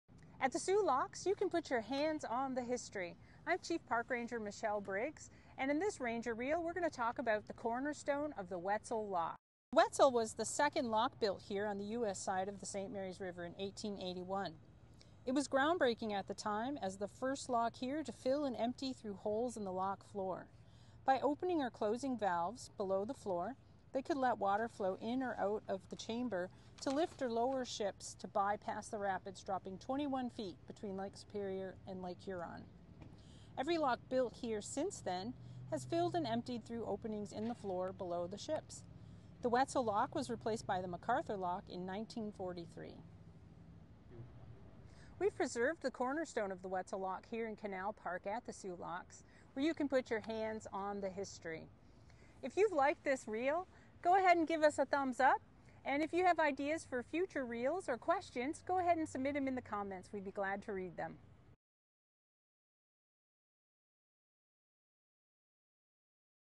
Ranger Reels: The Ground Breaking Weitzel Lock
In this Ranger Reel, Chief Park Ranger Michelle takes a look at the groundbreaking technology of the 1881 Weitzel Lock at the Soo Locks.